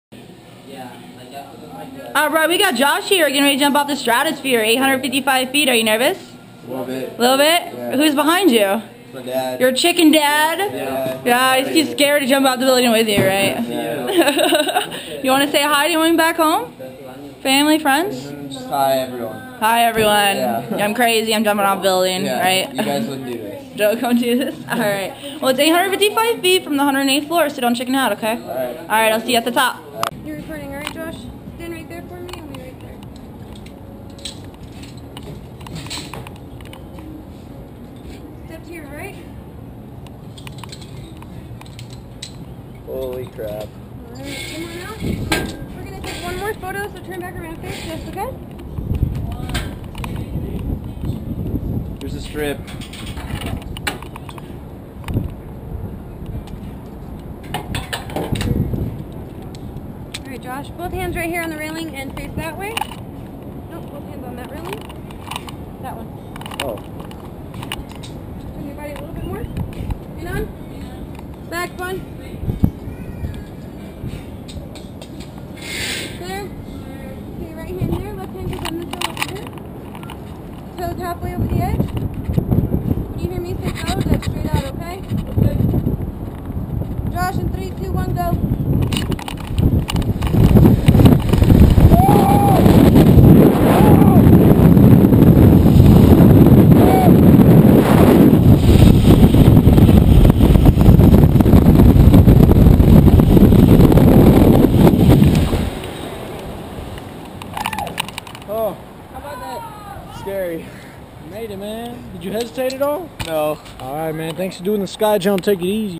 Josh Hull Jumping off the Stratosphere.
Josh Hull goes to Las Vegas, Nevada and jumps off the Sky Jump on the Stratosphere Tower, 855 feet above the ground.